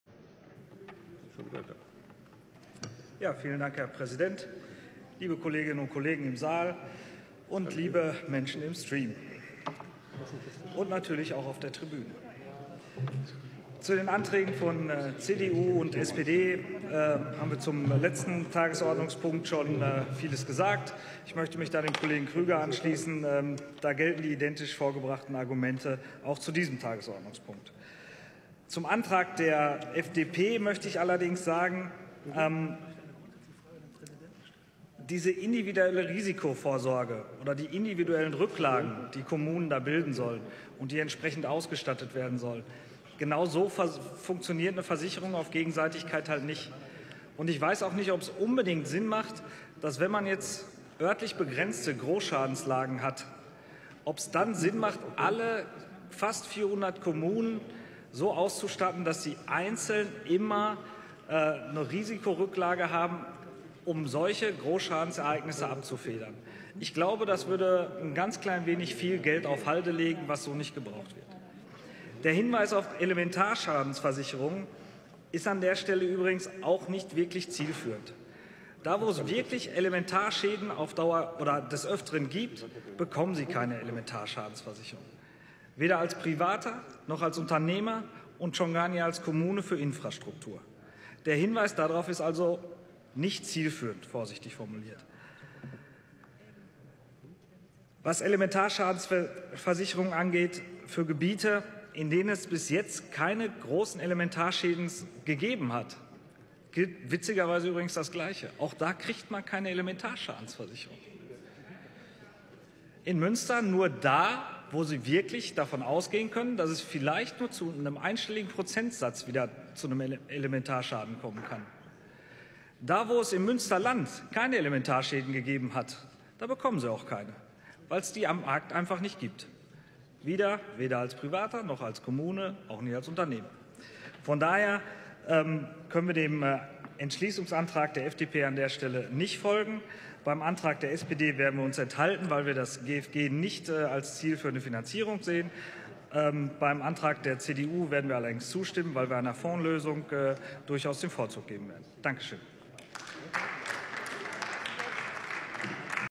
Torsten Sommer (@tosopiratas) zur Hilfe für die unwettergeschädigten Gemeinden im Münsterland
Donnerstag, 11. September 2014

Top 8. Nach dem Jahrhundertregen in Münster, Greven und dem Münsterland – Jetzt die betroffenen Städte und Gemeinde nicht allein lassen